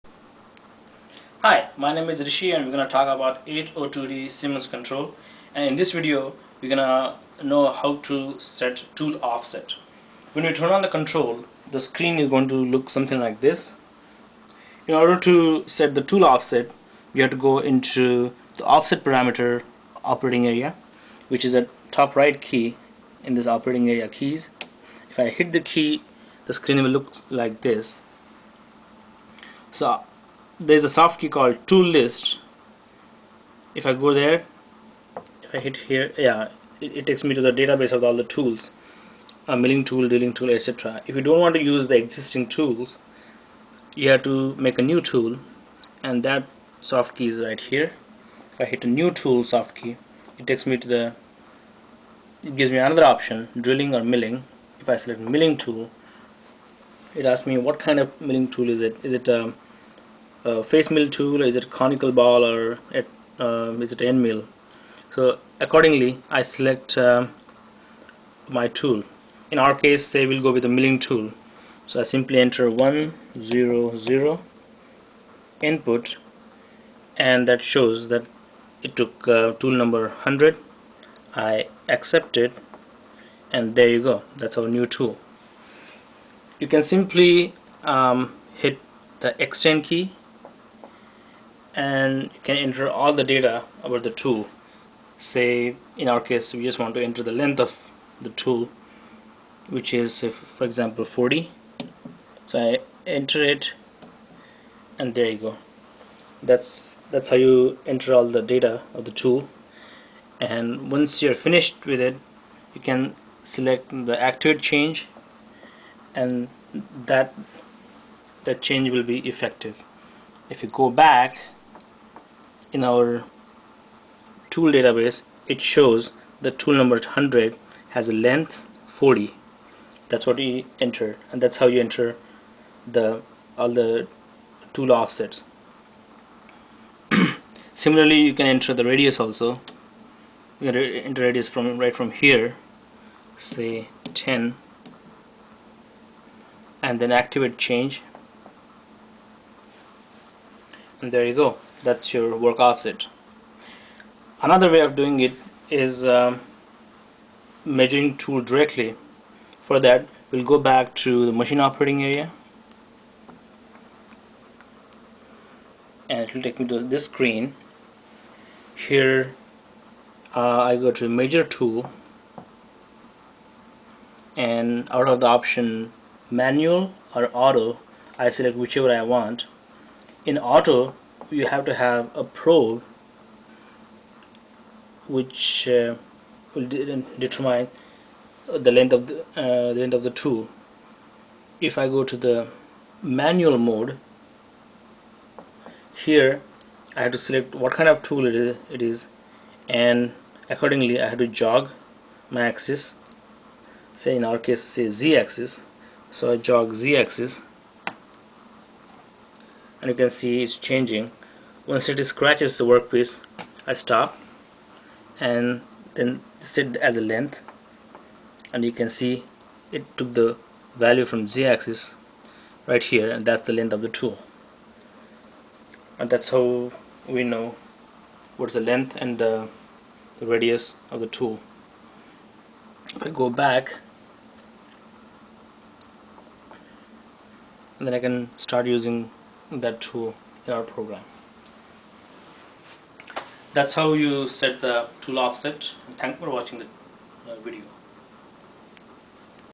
Tool offset.MPG
This video explains how to set tool offset values in SIEMENS 802D control.